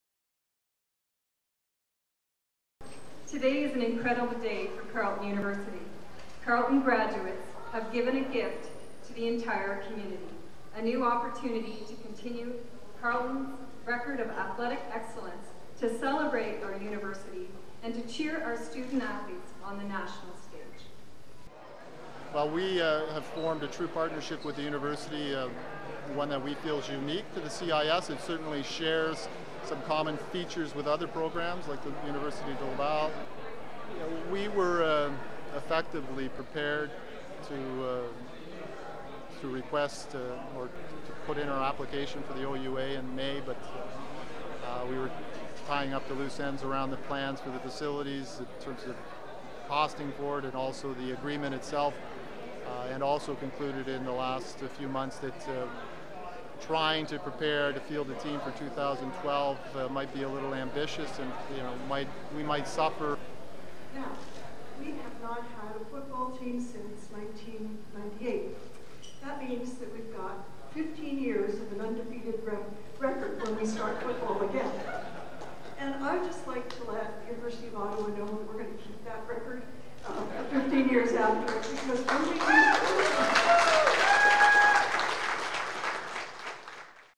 The Charlatan: Return of Ravens football
Carleton University held a press conference July 6, 2011, to announce the return of the Carleton Ravens men's football team in 2013.

Footage taken by Farhan Devji
Production by Melissa Schilz and Lewis Smith